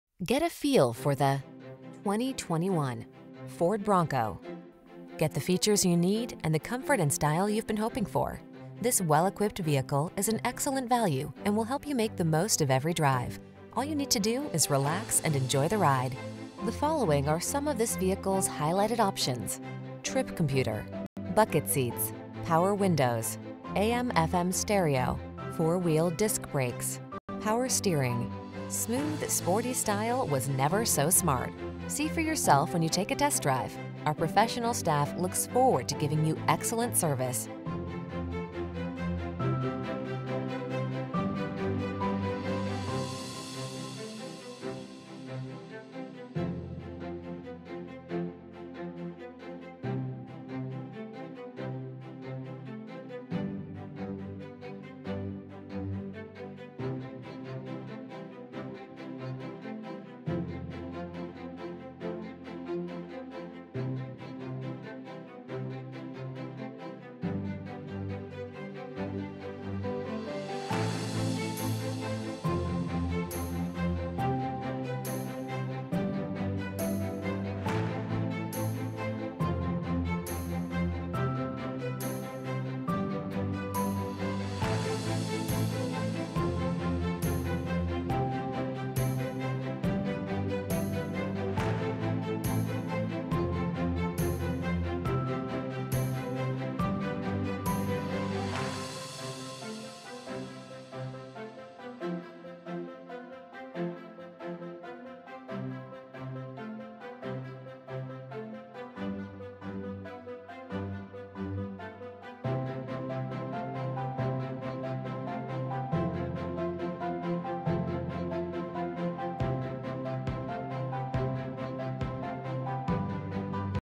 2021 Ford Bronco Jacksonville, Orange Park, Gainesville, Ocala, Lake City, FL MLA63068
Cyber Orange Metallic Tricoat New 2021 Ford Bronco available in Starke, Florida at Murray Ford Superstore. Servicing the Jacksonville, Orange Park, Gainesville, Ocala, Lake City, FL area.

13447 US Hwy 301 S